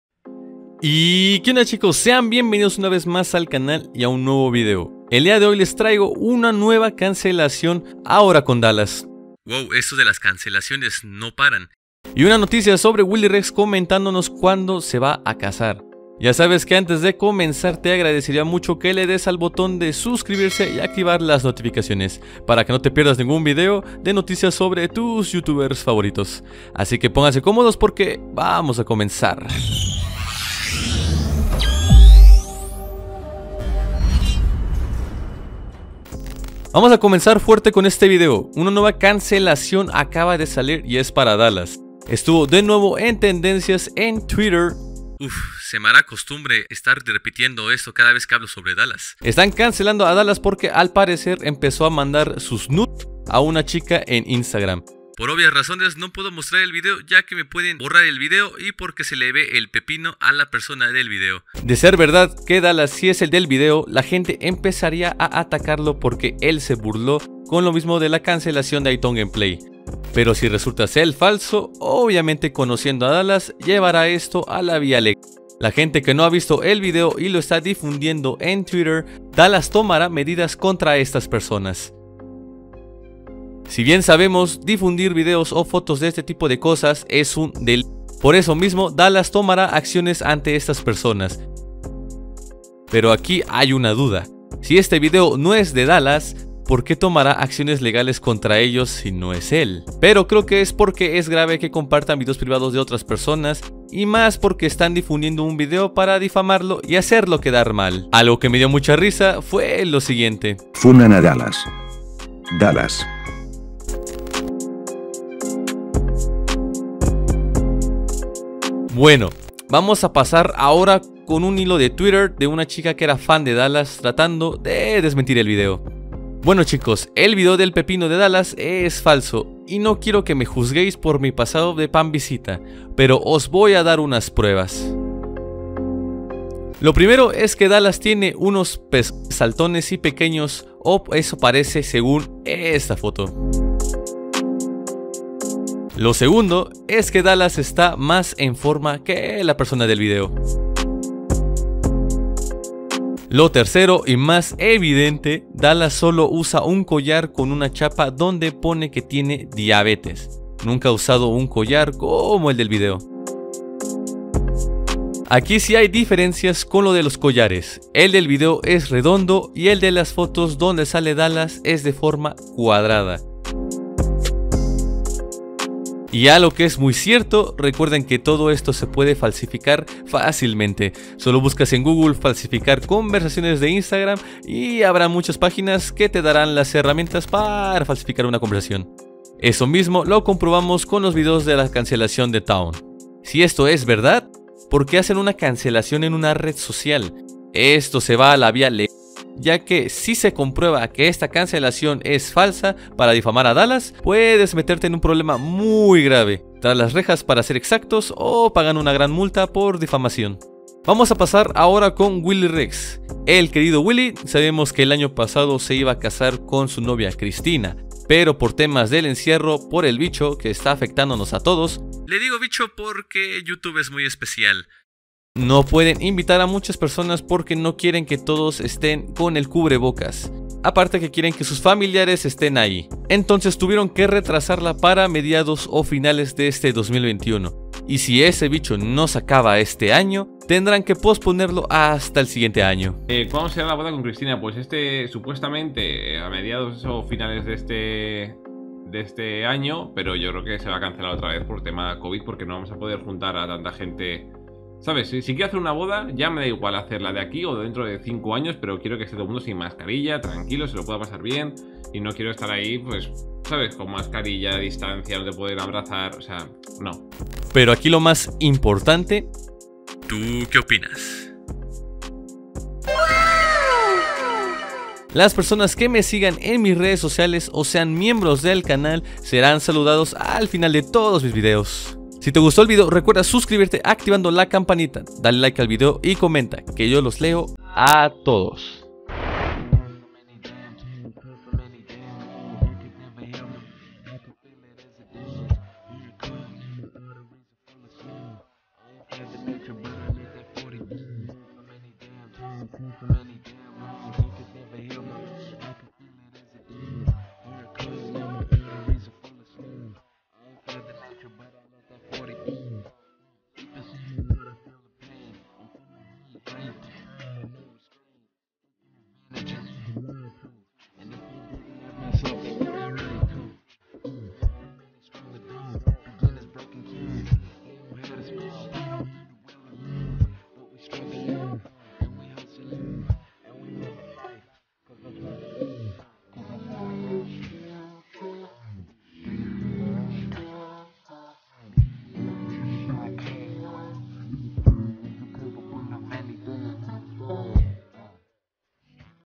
DALAS REVIEW CANCELADO POR SU P4CK | WILLYREX DICE CUANDO SE CASARÁ😱
Pero aquí los más importante...¿Tú que opinas?🤔